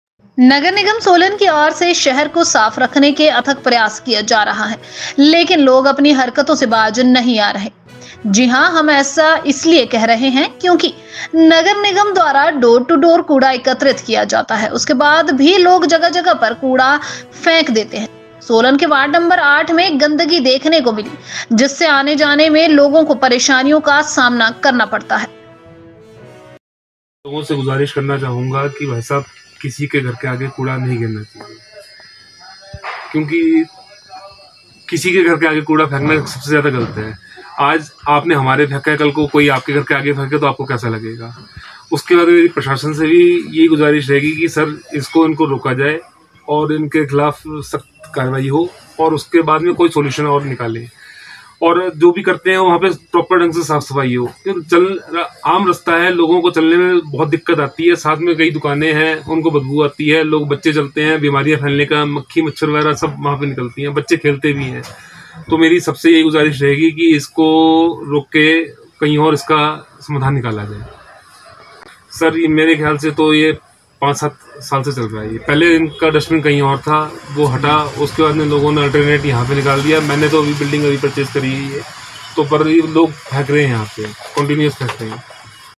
MC Solan | Clean | Garbage
Municipal Corporation Solan is making tireless efforts to keep the city clean but people are not desisting from their activities... Yes, we are saying this because the Municipal Corporation collects garbage door to door. Even after that, people throw garbage at various places. Dirt was seen in ward number 8 of Solan due to which people have to face problems while commuting.